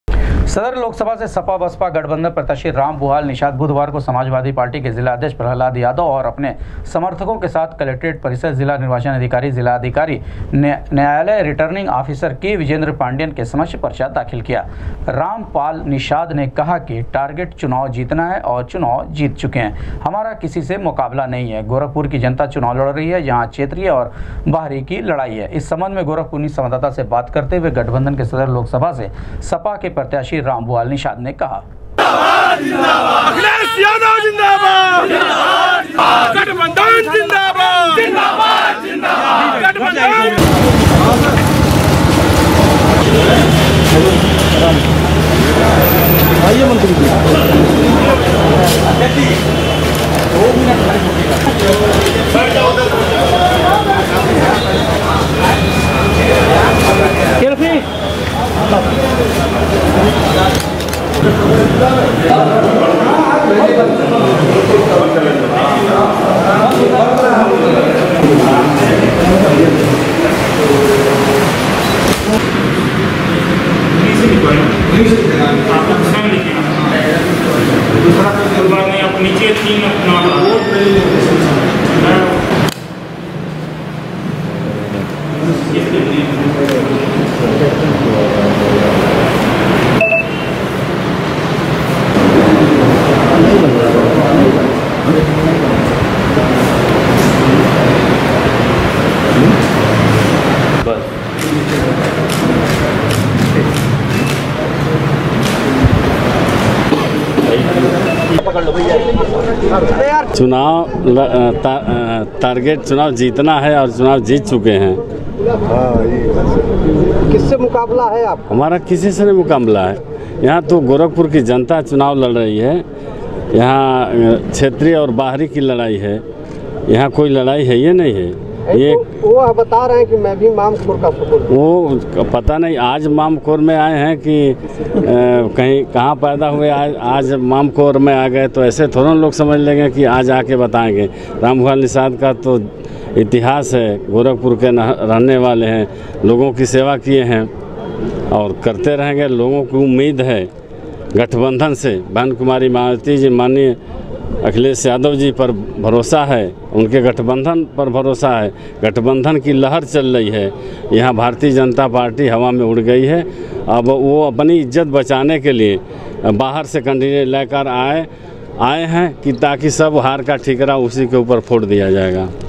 सदर लोकसभा से गठबंधन प्रत्याशी राम भुवाल निषाद ने किया नामांकन, कहा जीत चुके हैं चुनाव
सदर लोकसभा से गठबंधन प्रत्याशी राम भुवाल निषाद ने किया नामांकन, कहा जीत चुके हैं चुनाव, क्षेत्रीय और बाहरी की है लड़ाई